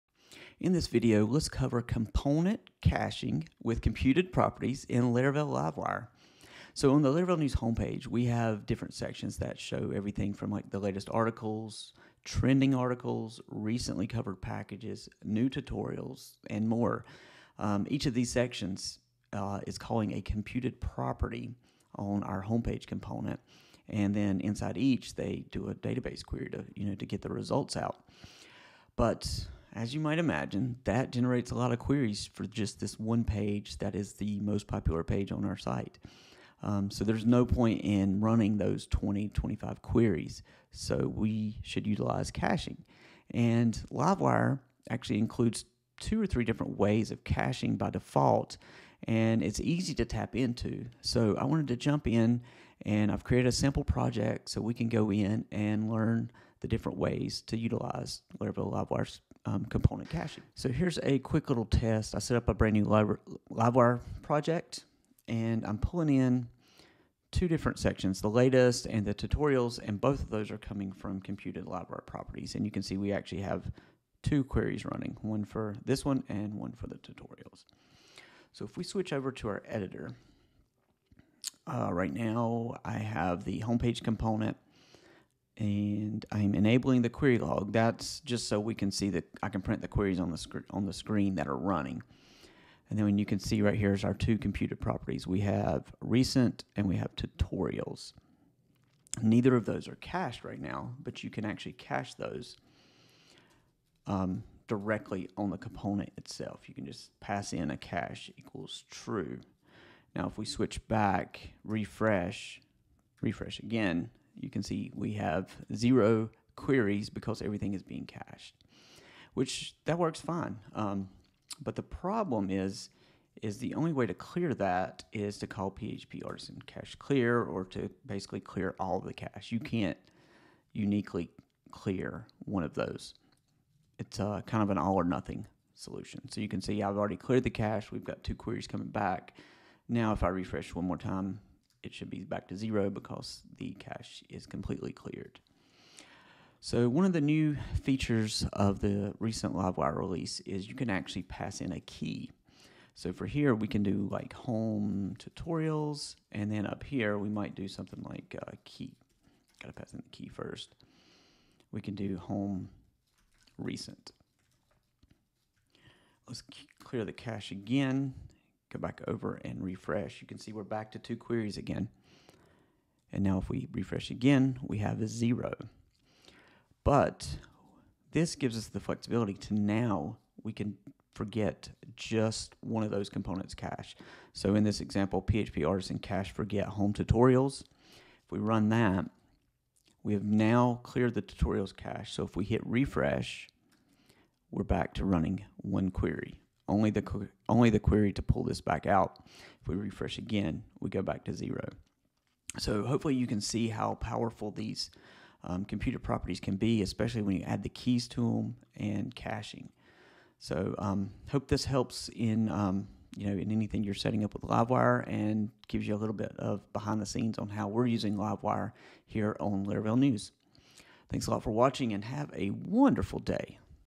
Laravel Livewire computed property caching with custom keys
Join us as we deep-dive into Laravel Livewire's component caching and computed properties. We'll share tips and tricks to optimize your database queries, clear caches, and improve your website's performance.

00:00 Introduction
01:24 Component Caching
02:01 Clearing Cache
02:56 Cache Keys
03:19 Selective Cache Clearing
03:37 Conclusion